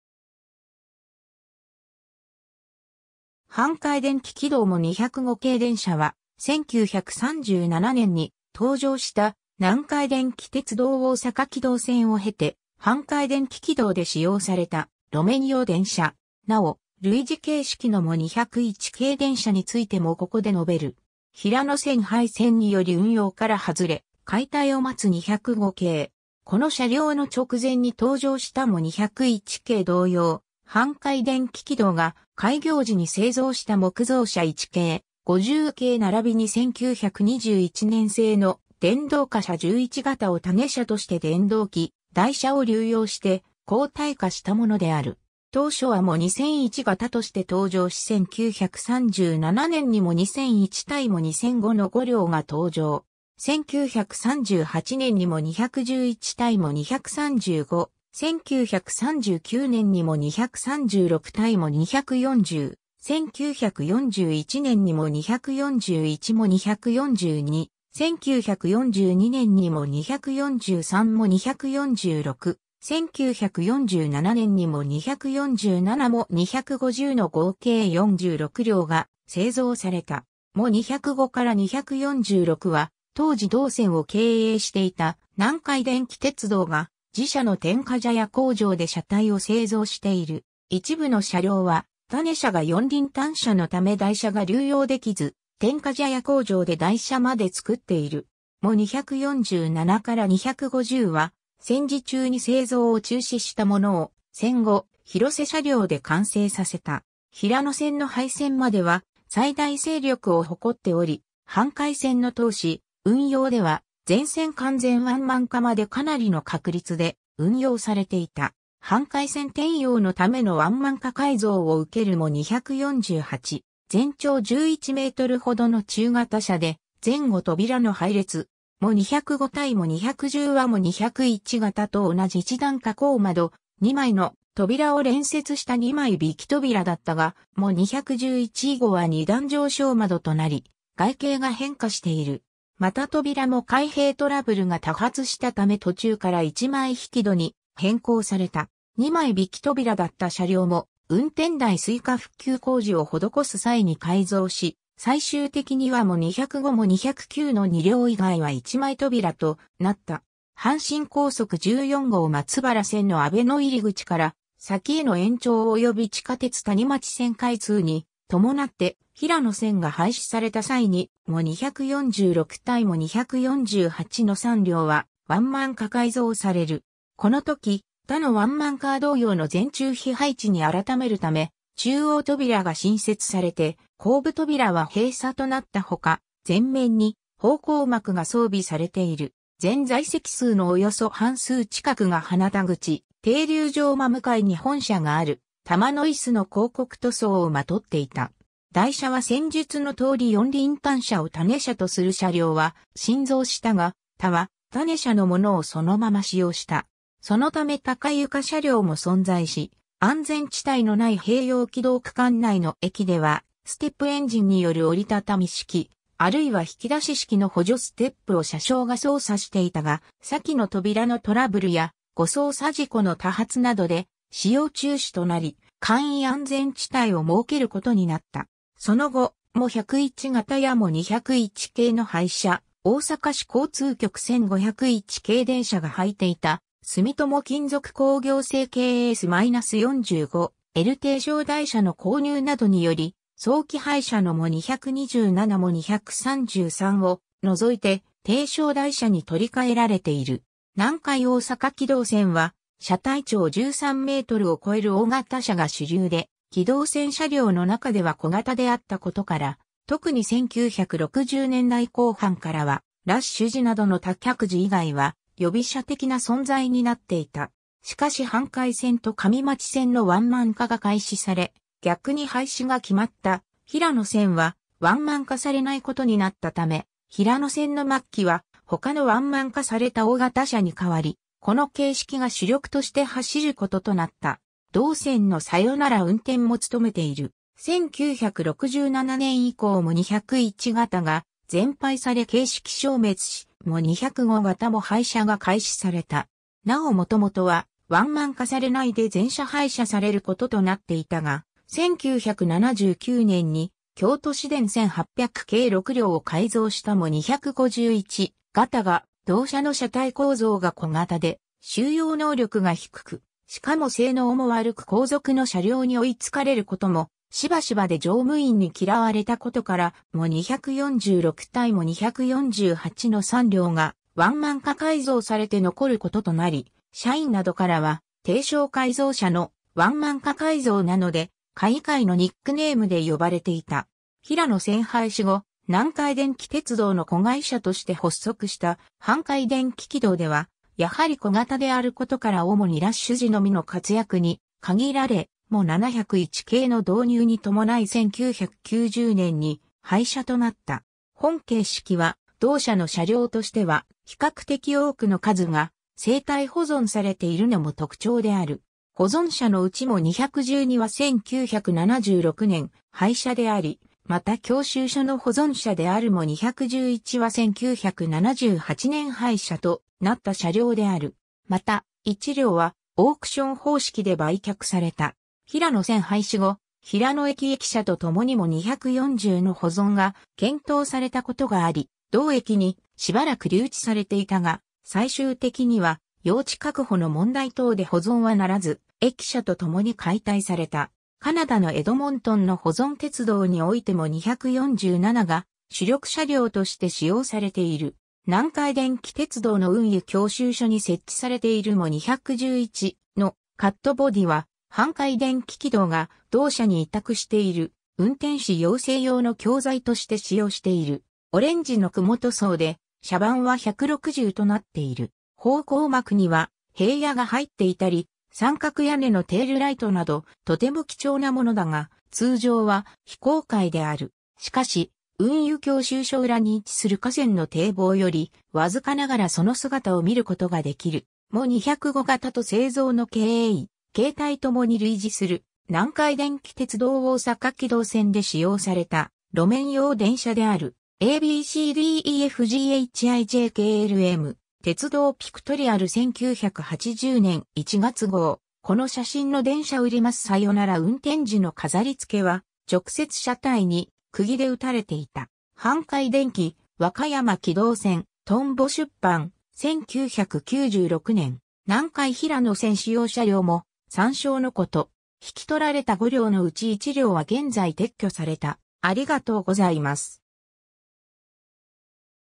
阪堺電気軌道205形電車
阪堺電気軌道モ205形電車（はんかいでんききどうモ205がたでんしゃ）は、1937年に登場した、南海電気鉄道大阪軌道線を経て、阪堺電気軌道で使用された路面用電車。
なお、類似形式のモ201形電車についてもここで述べる。
 平野線廃線により運用から外れ、解体を待つ205形。
 この車両の直前に登場したモ201形同様、（旧）阪堺電気軌道が開業時に製造した木造車1形・50形ならびに1921年製の電動貨車11形を種車として電動機、台車を流用して鋼体化したものである。
 当初はモ2001形として登場し1937年にモ2001 - モ2005（後にモ205 - モ210に改番）の5両が登場。
1938年にモ211 - モ235、1939年にモ236 - モ240、1941年にモ241・モ242、1942年にモ243 - モ246、1947年にモ247 - モ250の合計46両が製造された。
 モ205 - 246は当時同線を経営していた南海電気鉄道が、自社の天下茶屋工場で車体を製造している（モ232は戦災復旧車で、1949年に広瀬車両で車体を新造）。
一部の車両は種車が四輪単車のため台車が流用できず、天下茶屋工場で台車まで作っている。
モ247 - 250は戦時中に製造を中止したものを戦後、広瀬車両で完成させた。
 平野線の廃線までは最大勢力を誇っており、阪堺線の通し運用では全線完全ワンマン化までかなりの確率で運用されていた。
 阪堺線転用のためのワンマン化改造を受けるモ248。
 全長11メートルほどの中型車で、前後扉の配列。
モ205 - モ210はモ201形と同じ1段下降窓・2枚の扉を連接した2枚引き扉だったが、モ211以後は2段上昇窓となり、外形が変化している。
また扉も開閉トラブルが多発したため途中から1枚引き戸に変更された。
2枚引き扉だった車両も運転台垂下復旧工事を施す際に改造し、最終的にはモ205・モ209の2両以外は1枚扉となった。
 阪神高速14号松原線の阿倍野入口から先への延長及び地下鉄谷町線開通に伴って平野線が廃止された（1980年11月27日）際に、モ246 - モ248の3両はワンマン化改造される。
この時、他のワンマンカー同様の前中扉配置に改めるため、中央扉が新設されて後部扉は閉鎖となったほか、前面に方向幕が装備されている。
 全在籍数のおよそ半数近くが花田口停留場真向かいに本社があるタマノイ酢の広告塗装を纏っていた。
 台車は先述の通り4輪単車を種車とする車両は新造したが、他は種車のものをそのまま使用した。
そのため高床車両も存在し、安全地帯のない併用軌道区間内の駅では、ステップエンジンによる折りたたみ式、あるいは引き出し式の補助ステップを車掌が操作していたが、先の扉のトラブルや誤操作事故の多発などで使用中止となり、簡易安全地帯を設ける事になった。
 その後、モ101形やモ201形の廃車・大阪市交通局1501形電車が履いていた住友金属工業製KS-45L低床台車の購入などにより、早期廃車のモ227・モ233を除いて低床台車に取り替えられている。
 南海大阪軌道線は車体長13メートルを超える大型車が主流で軌道線車両の中では小型であったことから、特に1960年代後半からはラッシュ時などの多客時以外は予備車的な存在になっていた（1975年の時点では先の連接扉車である2両を含む7両が休車になっていた）。
しかし阪堺線と上町線のワンマン化が開始され、逆に廃止が決まった平野線はワンマン化されないことになったため、平野線の末期は他のワンマン化された大型車に代わり、この形式が主力として走ることとなった。
同線のさよなら運転も勤めている。
 1967年以降モ201形が全廃され形式消滅し、モ205形も廃車が開始された。
なお元々はワンマン化されないで全車廃車されることとなっていたが、1979年に京都市電1800形6両を改造したモ251形が、同車の車体構造が小型で収容能力が低く、しかも性能も悪く後続の車両に追い付かれる事もしばしばで乗務員に嫌われた事から、モ246 - モ248の3両がワンマン化改造されて残る事となり、社員などからは「低床改造車」の「ワンマン化改造」なので「カイカイ（改改）」のニックネームで呼ばれていた。
 平野線廃止後、南海電気鉄道の子会社として発足した阪堺電気軌道では、やはり小型である事から主にラッシュ時のみの活躍に限られ、モ701形の導入に伴い1990年に廃...